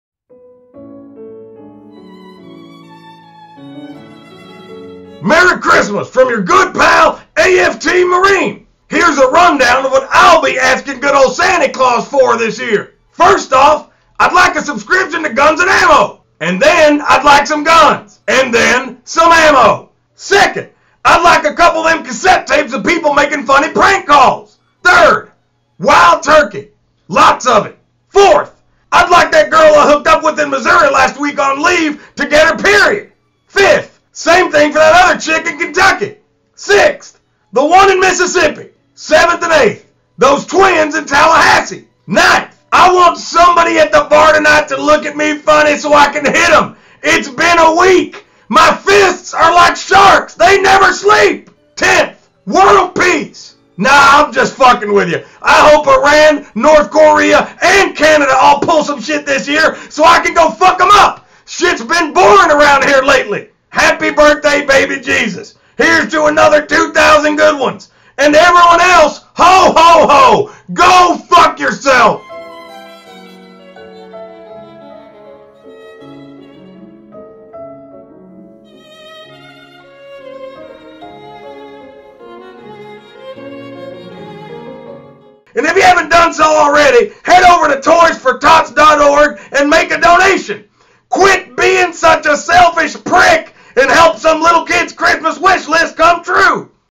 Marine's Christmas List - Guns, Ammo, Prank Call Tapes, Wild Turkey & More
Action Figure Therapy’s Marine reads off his Christmas list which includes guns, ammo, prank call tapes, wild turkey and other coveted items.

He also asks Santa to bring a period to random girl he hooked up with on leave, which sounds kind of like a tall order.